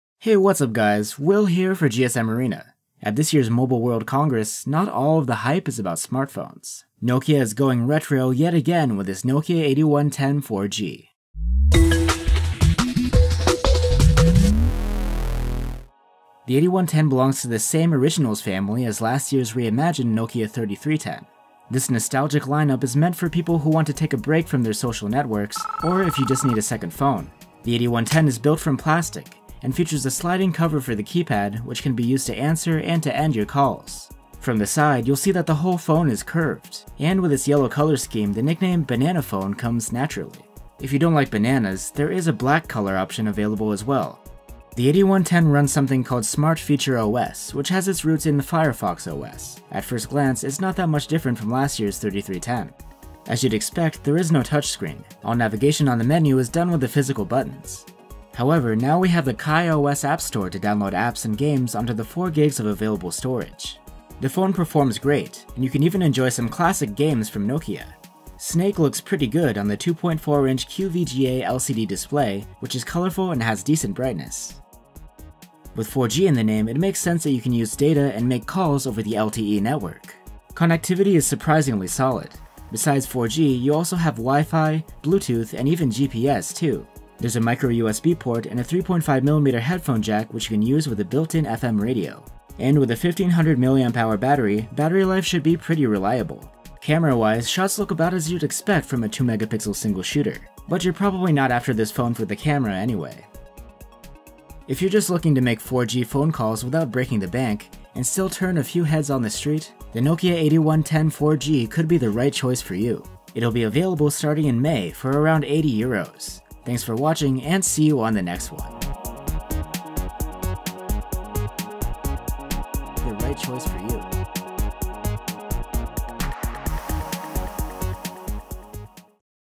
Nokia 8110 4G hands-on review
Our FIRST LOOK at the Nokia 8110 4G's top features, build and screen quality, performance, and camera image quality.